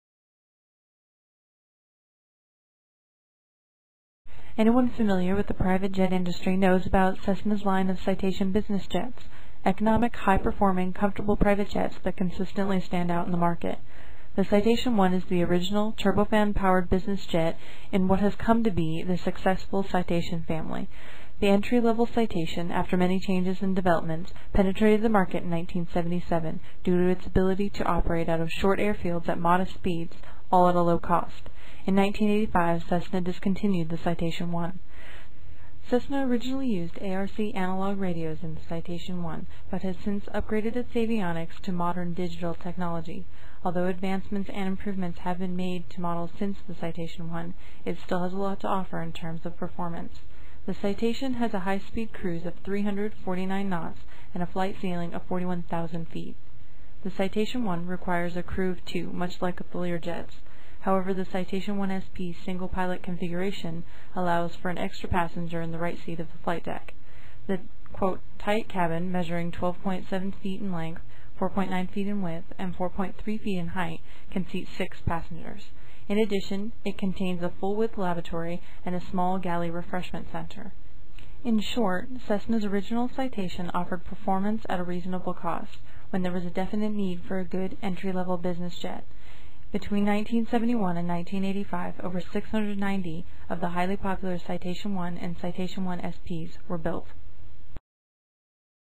Citation 1
Description of the Citation 1
These Performance and Comfort graphs by Jet Advisors demonstrate comparisons to other aircraft in the Compact Light Jet class.
BEST IN CLASS-Full Green
ABOVE AVERAGE-Part Green with Black Bar
BELOW AVERAGE-Part Amber with Black Bar
LOWEST IN CLASS-Full Amber